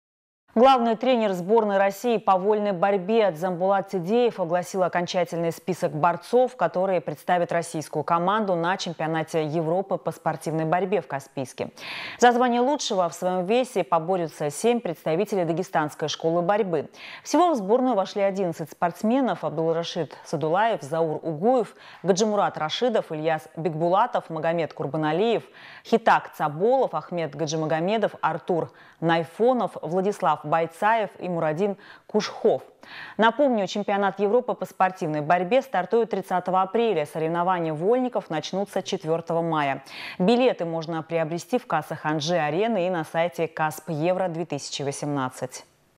Стали известны имена борцов, которые представят Россию на ЧЕ по спортивной борьбе в Каспийске
Главный тренер сборной России по вольной борьбе Дзамболат Тедеев огласил окончательный список борцов, которые представят российскую команду на чемпионате Европы по спортивной борьбе в Каспийске.